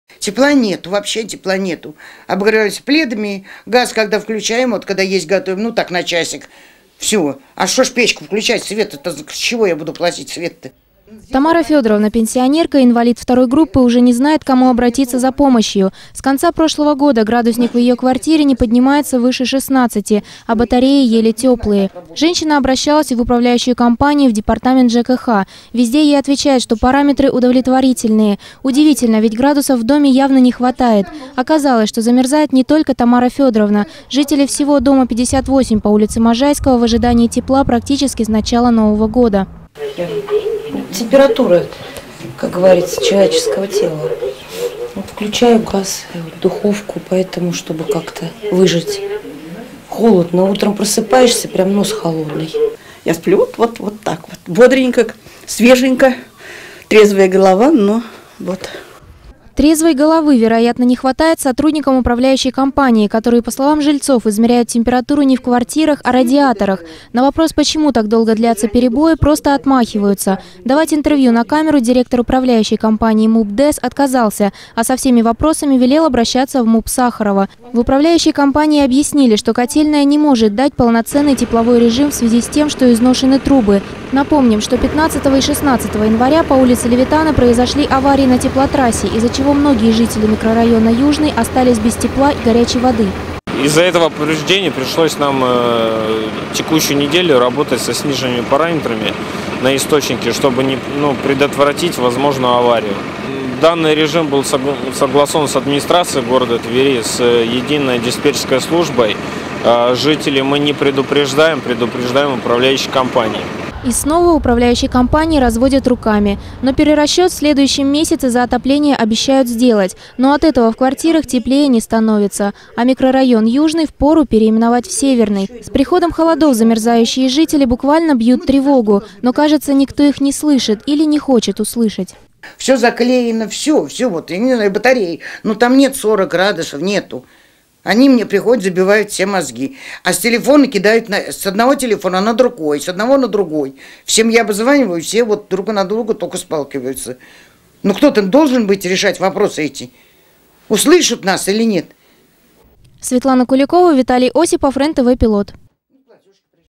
Борьба за тепло. 2015-01-23
Середина зимы, а многие жители Твери замерзают в своих квартирах. Среди них пенсионеры, инвалиды и дети – люди, кому просто необходимо тепло. Они вынуждены обогревать комнаты специальными приборами и держать включенным газ. Между тем в управляющей компании, на вопросы жителей просто разводят руками.